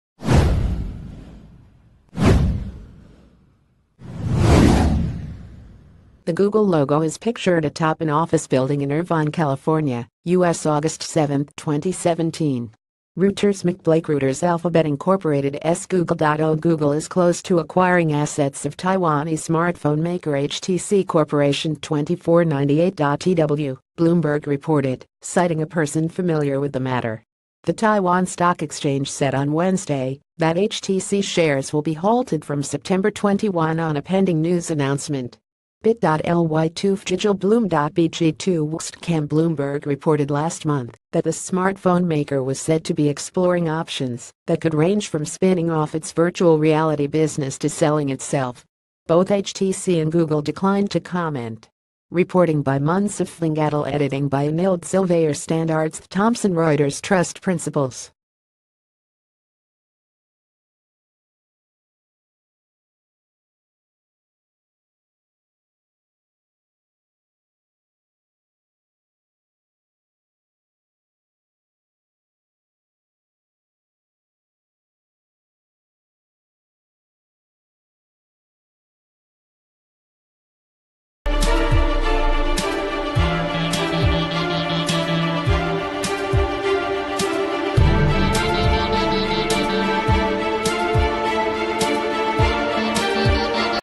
Breaking News | Google close to buying smartphone maker htc's assets: bloomberg
Google close to buying smartphone maker htc's assets: bloomberg

The Google logo is pictured atop an office building in Irvine, California, U.S. August 7, 2017. REUT...